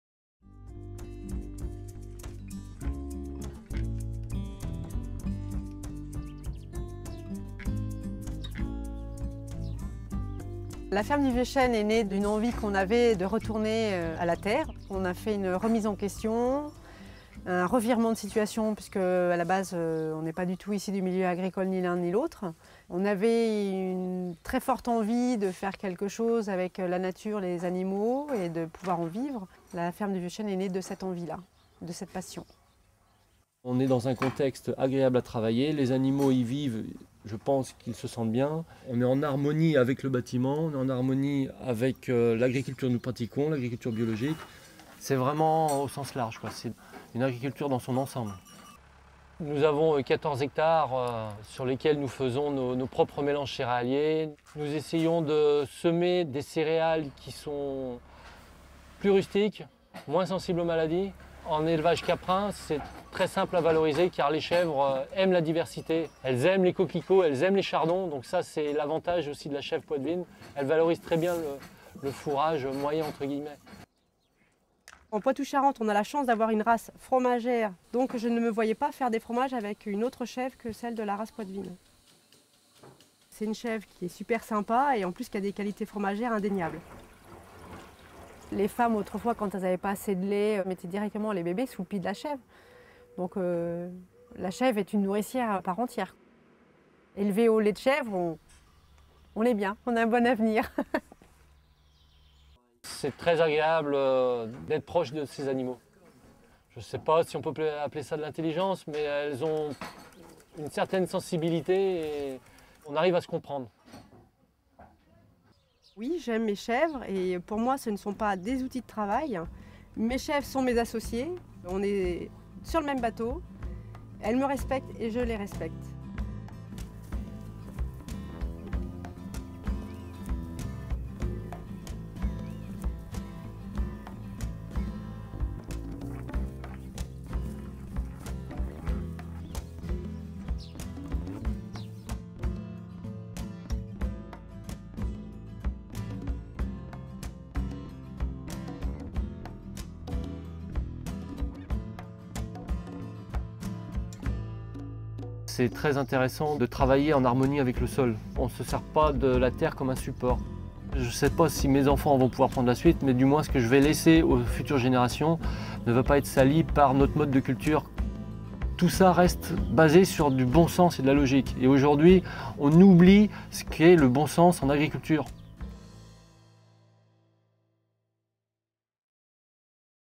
La chèvre Poitevine
Cette vidéo traite de l'élevage de la chèvre Poitevine dans une exploitation Biologique des Deux Sèvres. Polyculture et élevage dans le respect des animaux.